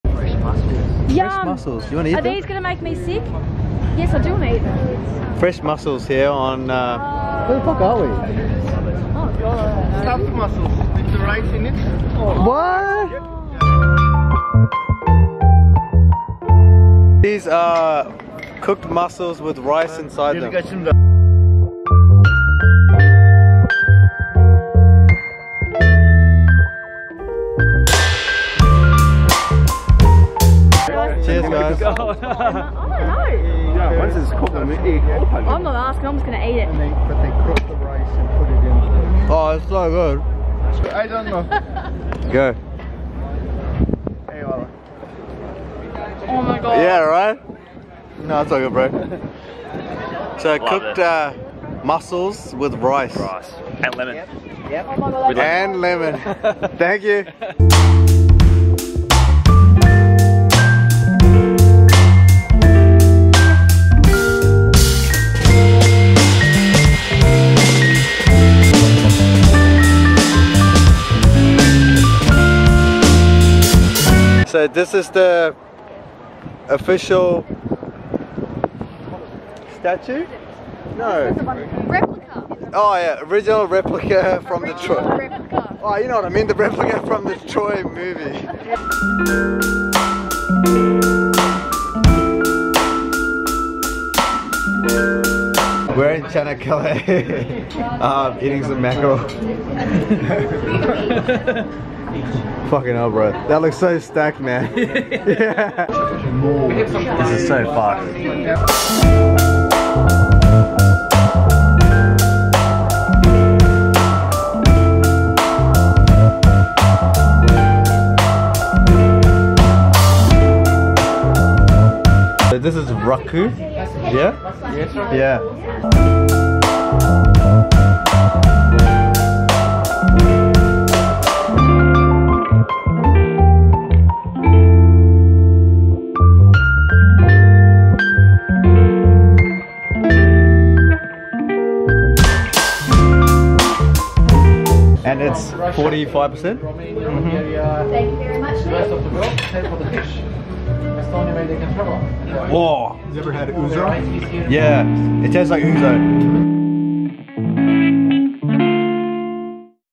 Turkey Episode 5 - Canakkale Food
The wooden horse is a replica of the original one from the Troy movie.
We ate some street mussels (which was a common street food - cooked mussels with rice), could not find out how they cooked the mussels with the rice some how?
After which we ate mountains of local fish for dinner with raki (a common  sweetened, often anise-flavoured, alcoholic drink, which is also 45% ALC content).